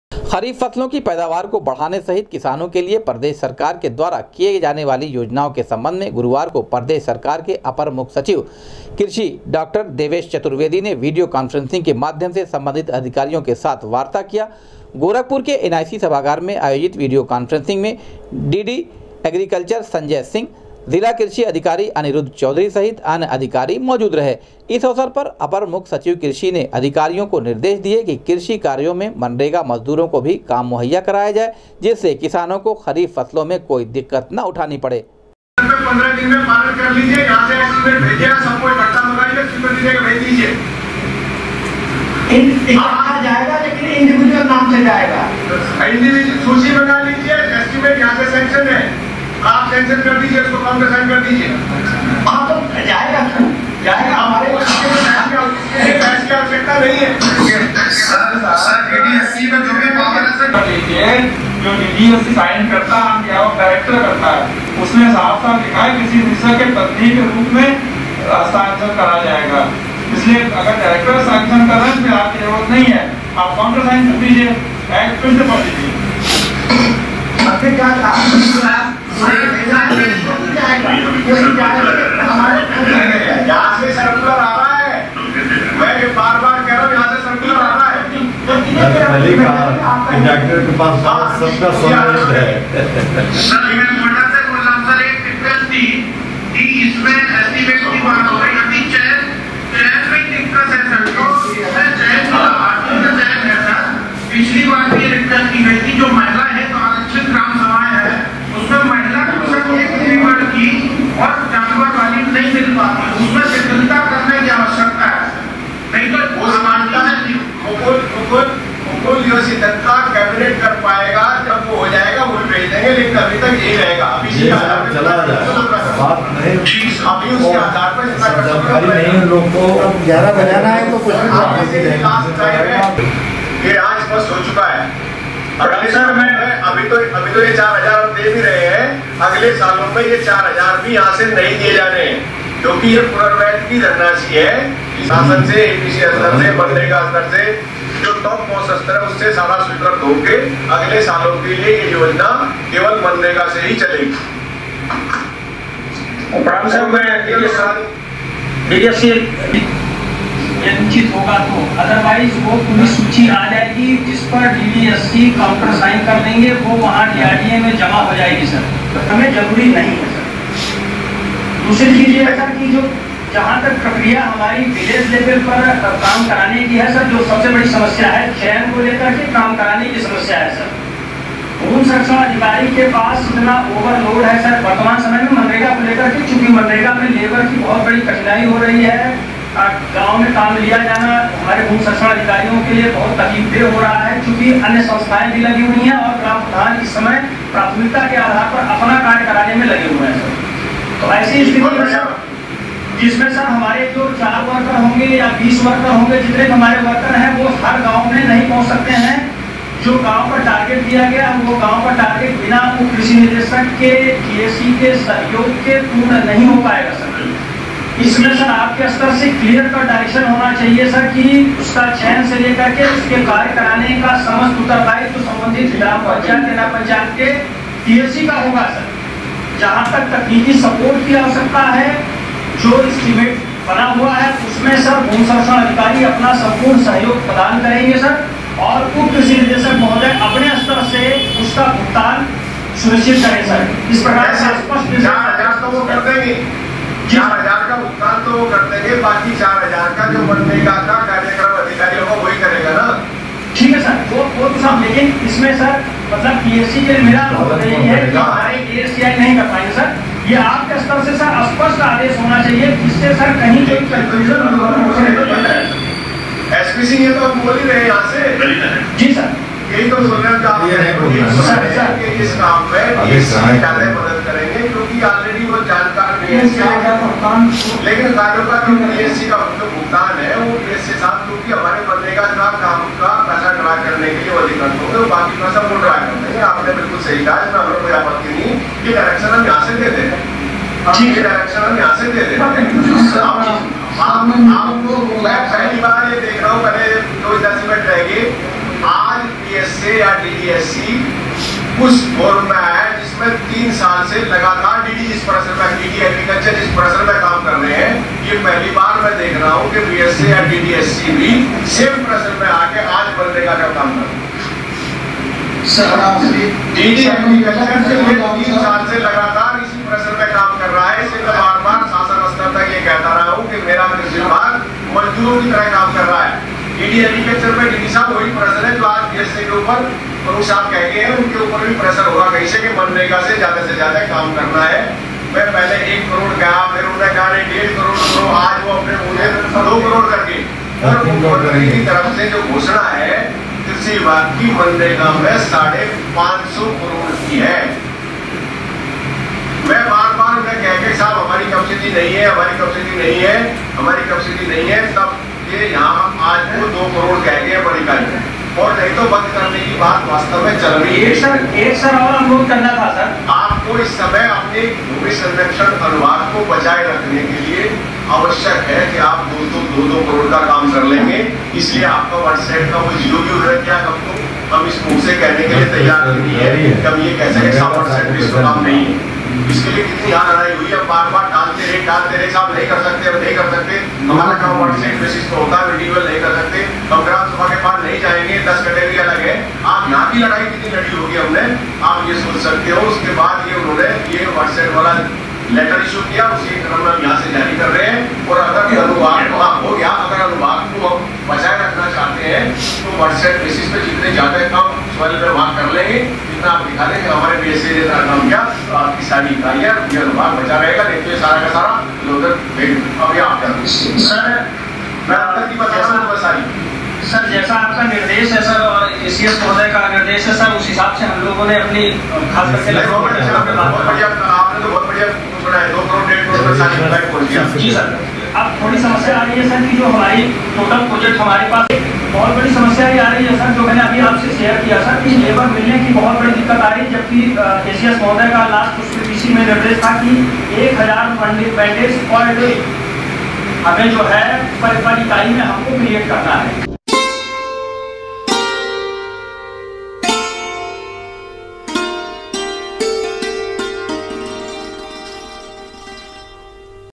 खरीफ फसलों के पैदावार को बढ़ाने सहित किसानों के लिए प्रदेश सरकार के द्वारा लागू योजनाओ के सम्बन्ध में
खरीफ फसलों के पैदावार को बढ़ाने सहित किसानों के लिए प्रदेश सरकार के द्वारा लागू योजनाओ के सम्बन्ध में अपर मुख्य सचिव कृषि ने वीडियो कांफ्रेंसिंग के माध्यम सम्बन्धित अधिकारियो के साथ की वार्ता, दिए आवश्यक दिशा निर्देश